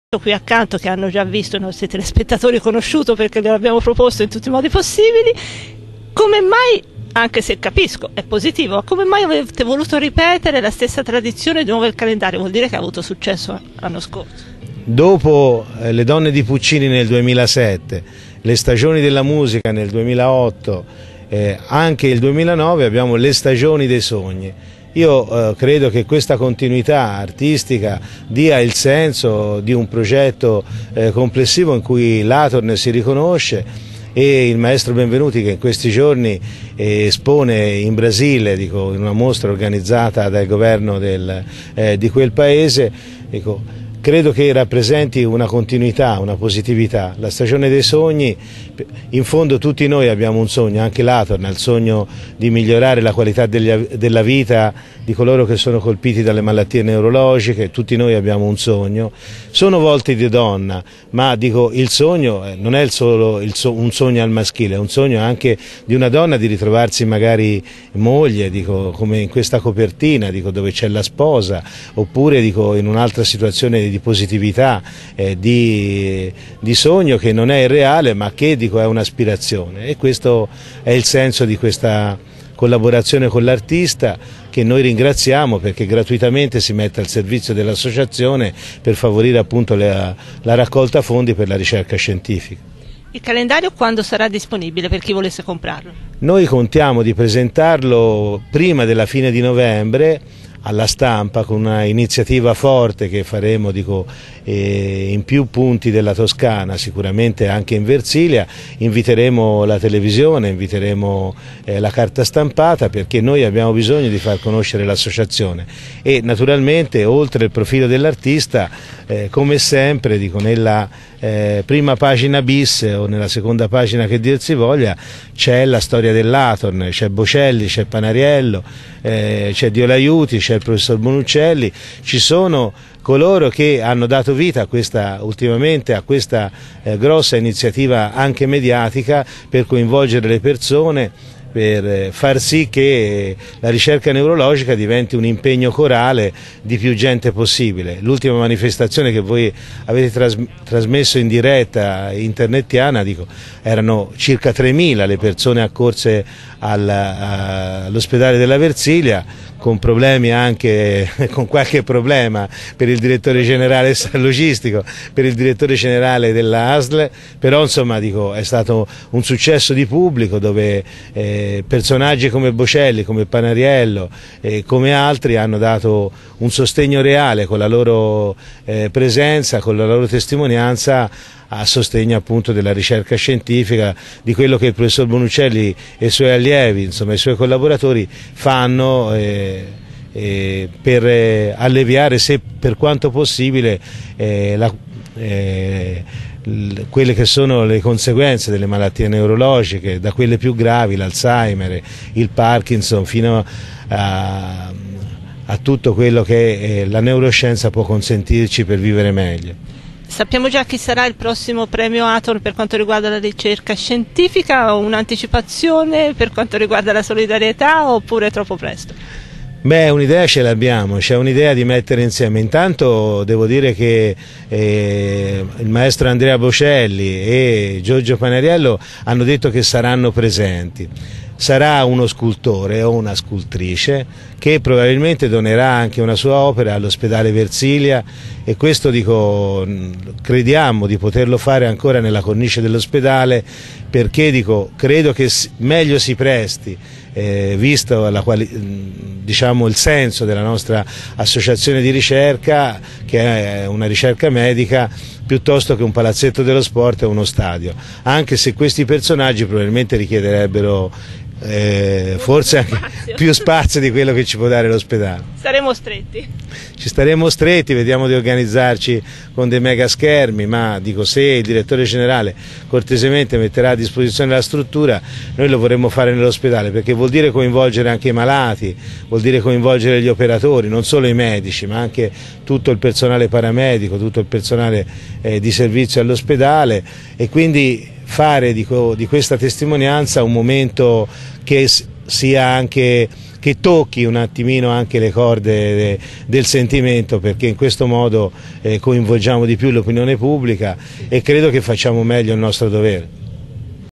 ANTEPRIMA : IL CALENDARIO ATORN 2009
Vediamo in anteprima il Calendario Atorn 2009. "LE STAGIONI DEI SOGNI" del maestro Riccardo Benvenuti, una celebrazione delicata dell'universo femminile e dei suoi sogni. Il ricavato dalla vendita del calendario verrà devoluto alla ricerca scientifica, missione in cui l'ATORN (Associazione Toscana per la ricerca neurologica) è impegnata da sempre. Ascoltiamo nell'intervista di Rosella Brogi il Presidente Atorn ,Gianfranco Antognoli.